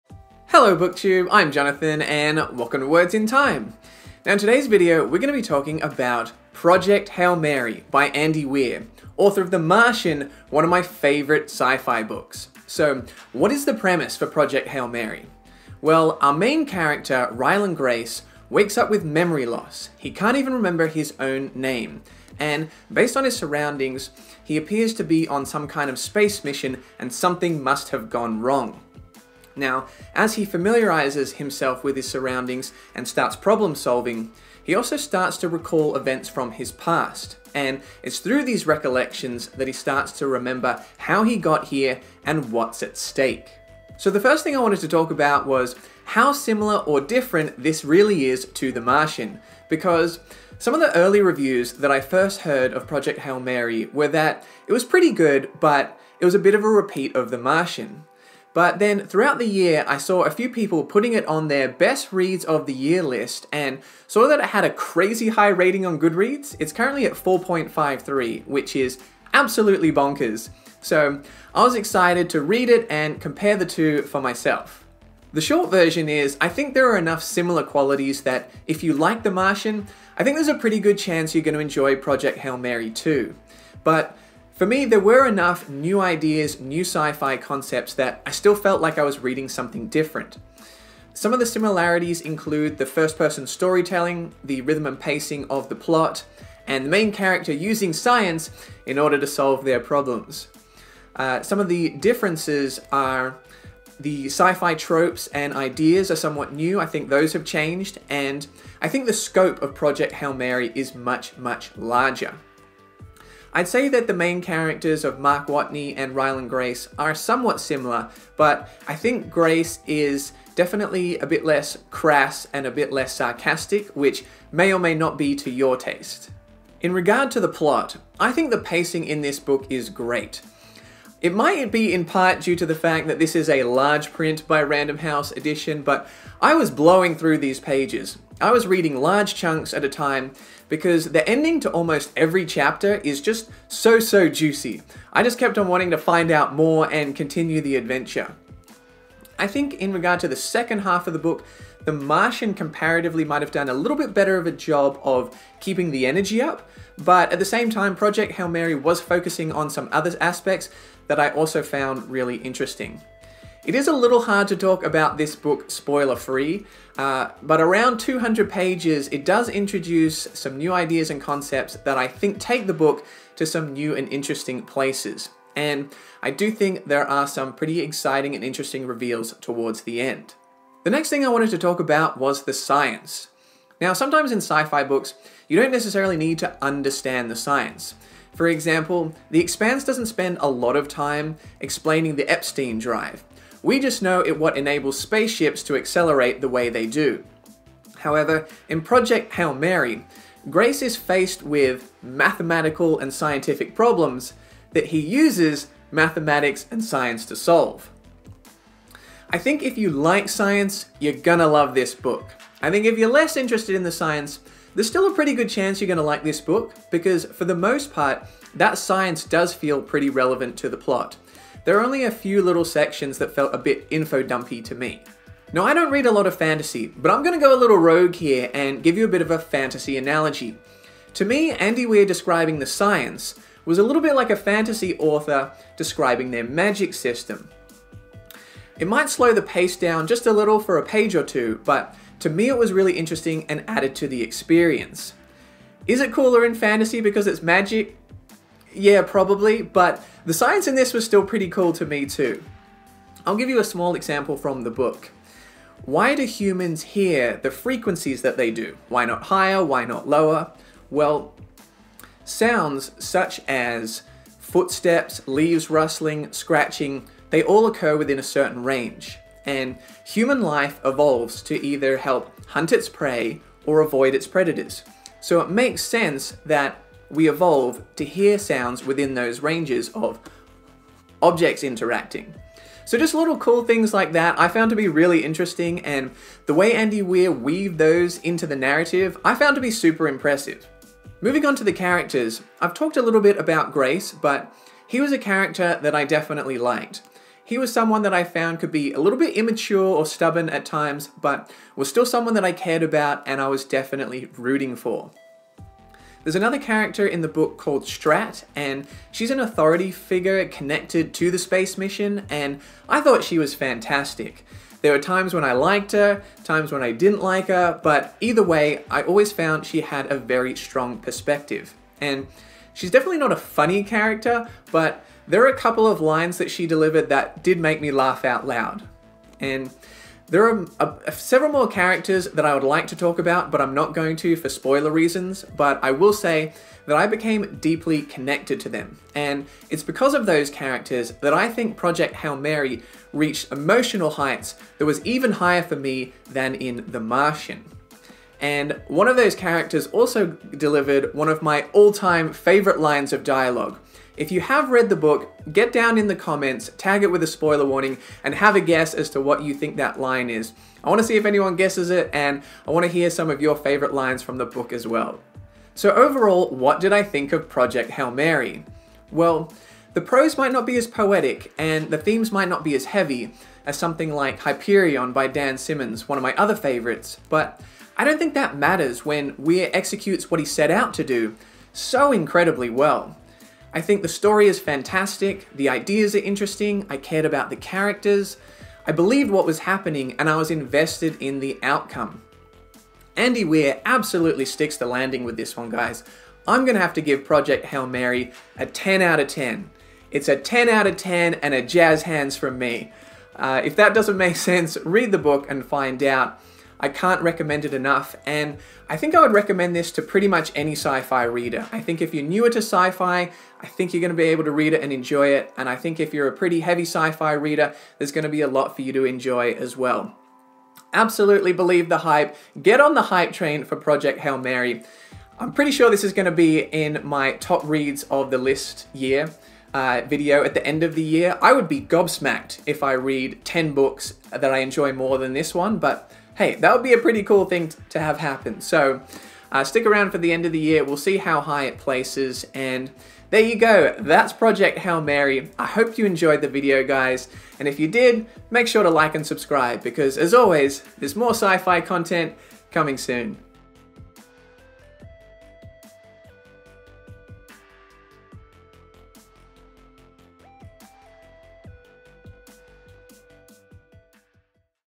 PROJECT HAIL MARY by ANDY WEIR | Sci-Fi Book Review
Andy Weir delivers another fast-paced space adventure with Project Hail Mary. But how does it compare to one of my favourite science fiction books, The Martian?